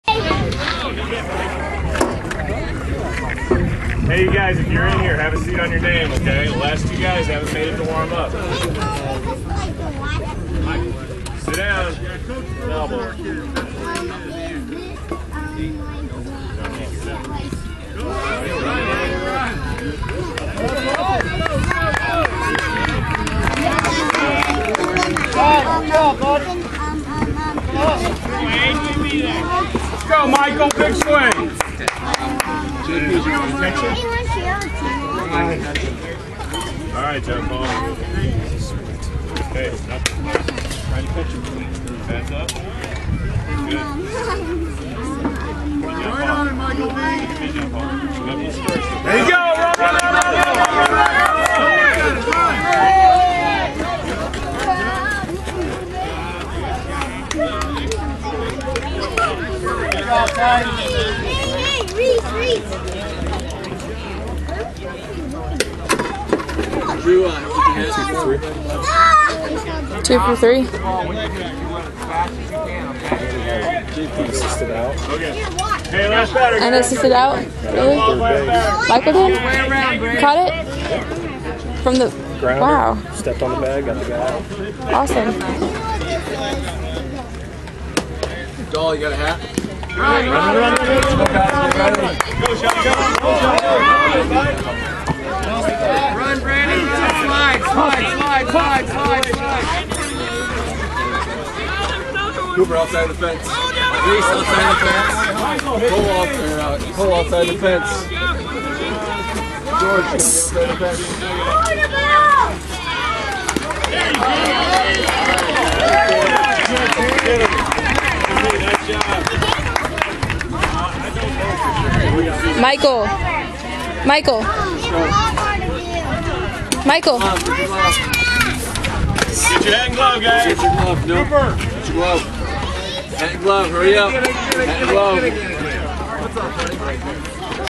Run Home and Tag the Base Any Way You Can!
Criss Castle's 5 year old grandson Michael (#13) has his own way of getting to home plate!